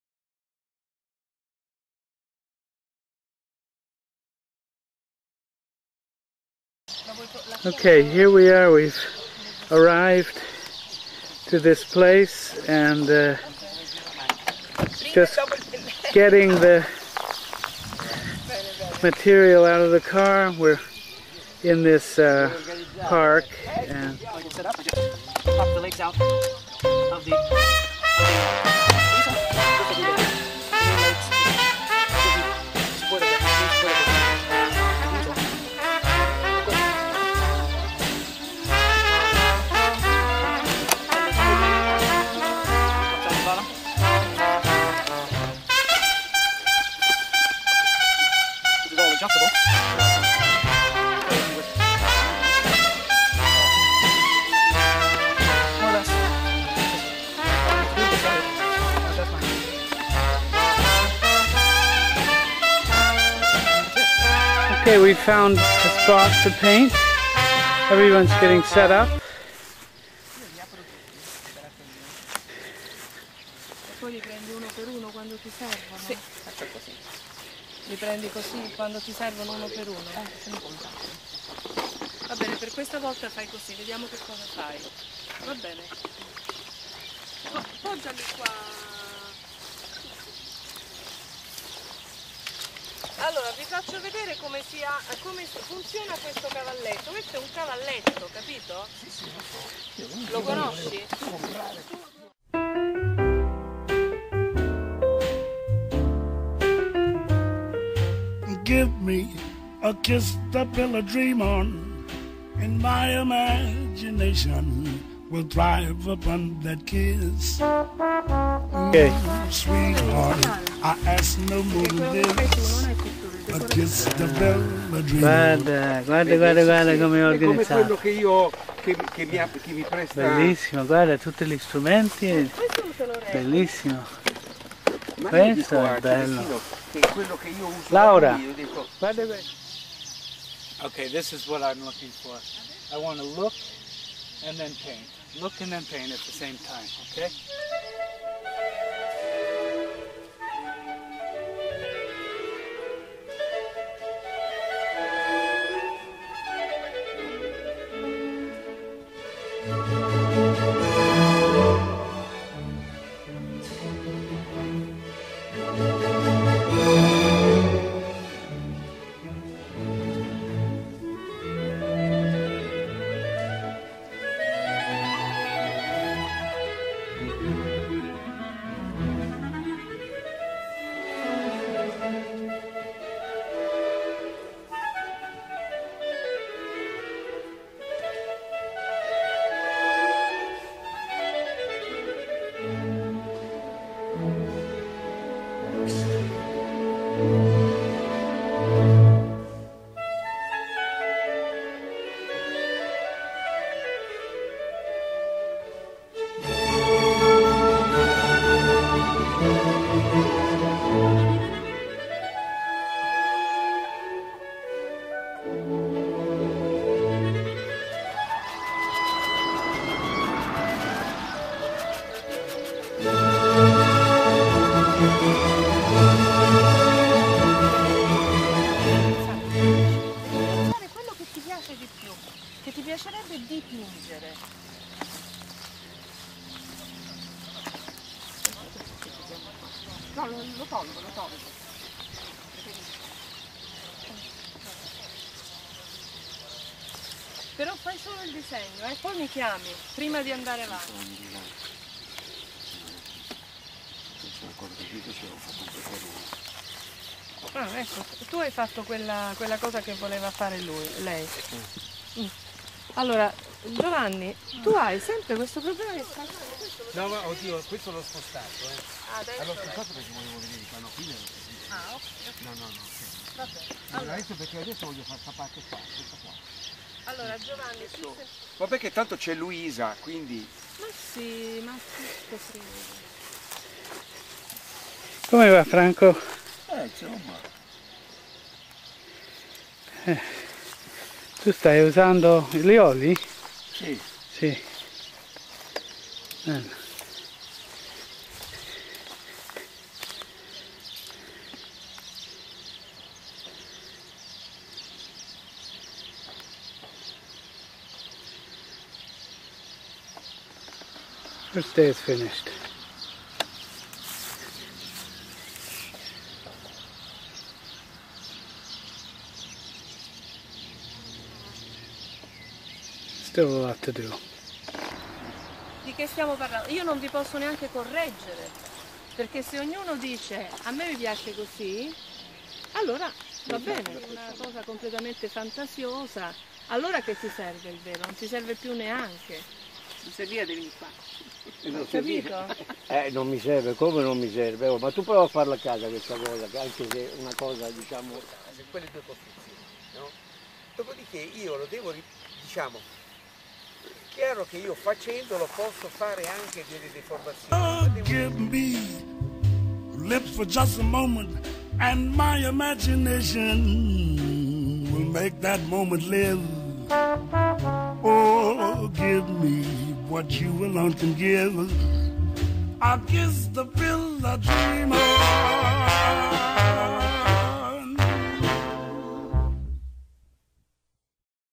A Morning En Plein Air
A group of En Plein Air painters go into the Roman countryside for a morning of painting. The Marcigliana Reserve is in the City of Rome just outside the ring road.